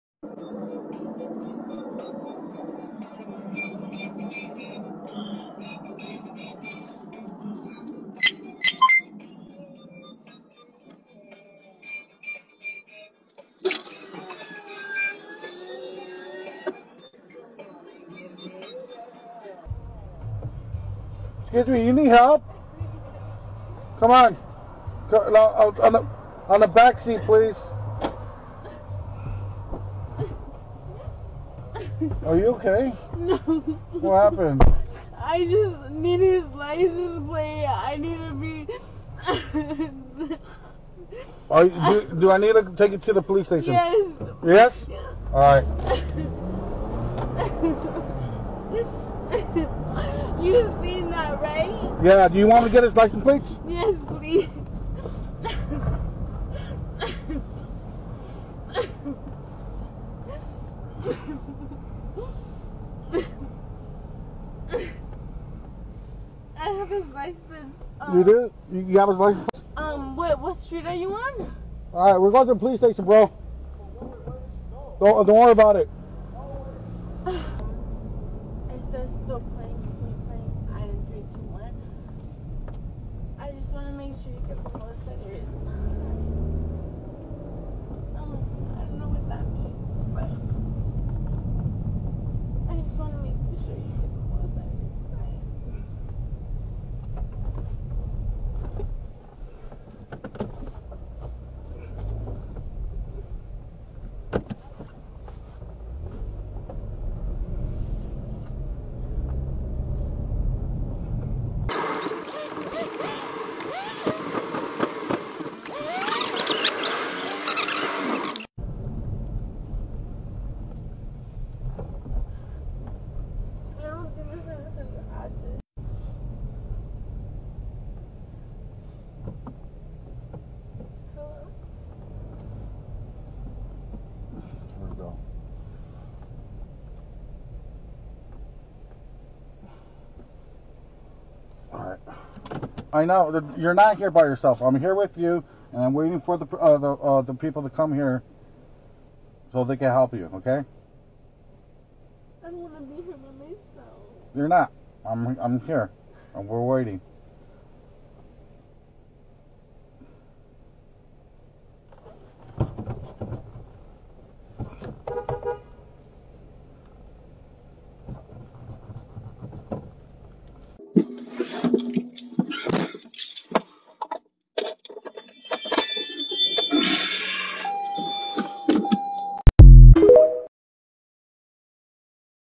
Someone jump out of a car
Working Uber and saw a female jump out of a moving car